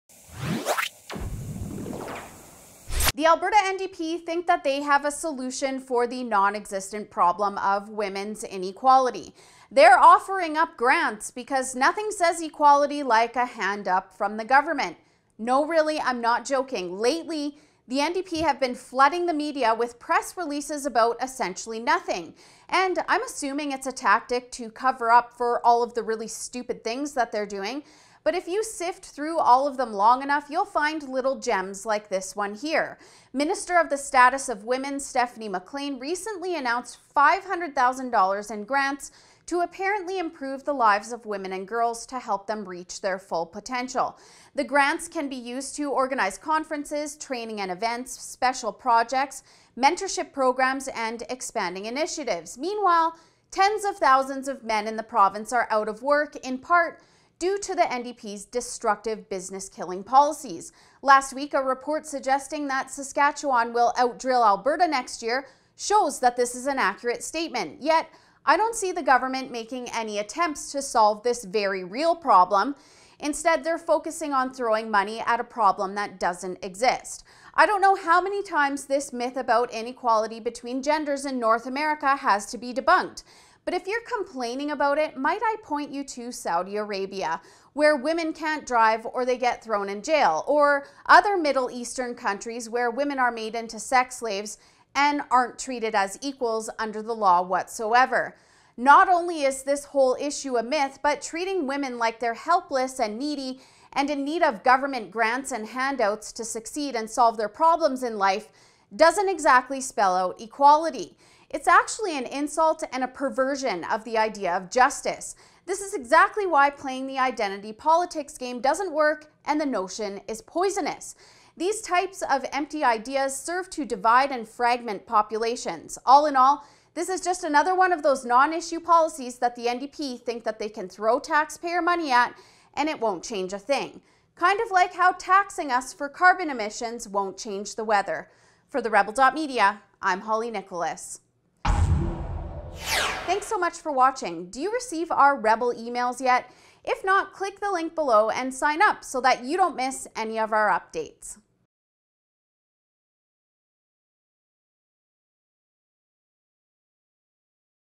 Notley NDP throw $500K at non-existent “problem” of women’s equality
Holly Nicholas reports NDP's Status of Women Minister announced $500,000 in grants to fix mythical gender inequality while ignoring the thousands of men in the province who are jobless due to destructive NDP policies. MORE